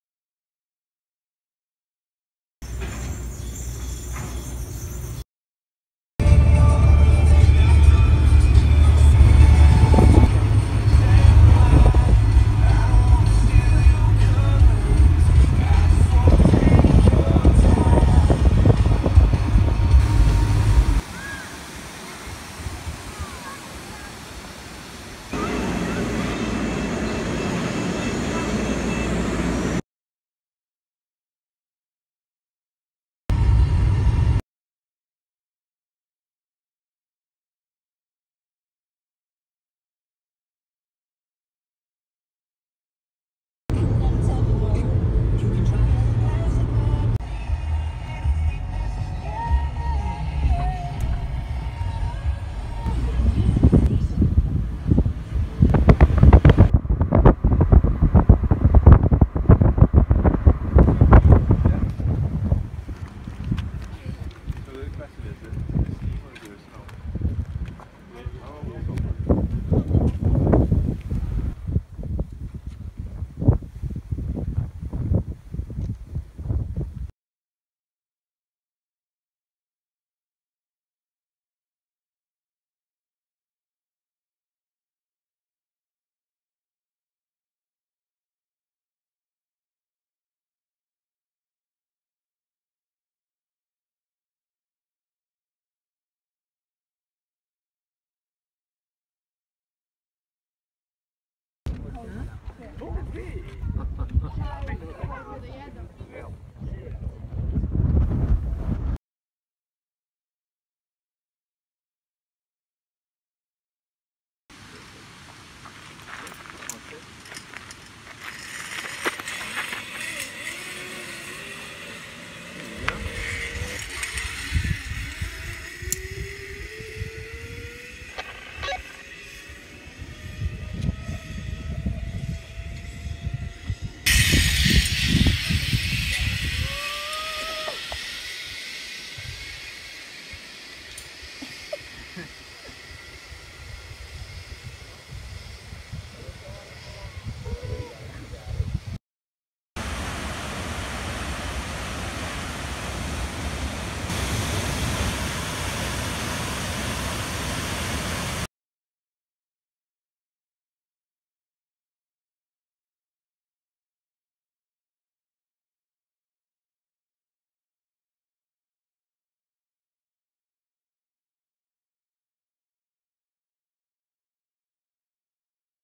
Places to go in Big Island, Hawaii!
August 2016 week long getaway! 👫❤️🏝

Stay active. Keep your inner fire burning. It's ok to let off steam. Have a blast! -Advice from a VOLCANO

Hale Ike at Hilton Waikoloa- perfect if you have kids, wanted to stay in one place or do not want to rent a car!

Atv Ride the rim-Awesome activity for adults! Get muddy and see wonderful sights!

Waipi'o Valley Falls

KMC cabin at Volcano- Did not like it here. It's rotten and old but if you're in military and wanted to save cash while staying in the island, this is a cheap stay!

Volcano National Park:
Jaggar Museum
Lava Tube
Steam vents
Crater Rim

Skyline Eco Adventures Zipline-Akaka Falls - 7 line zip lines!

Holiday Inn Express at Kona- Great price for the place. It's in the middle of beach and restaurants.

Frangini Mau Ola Spa- They have great groupon deal.

Green sand beach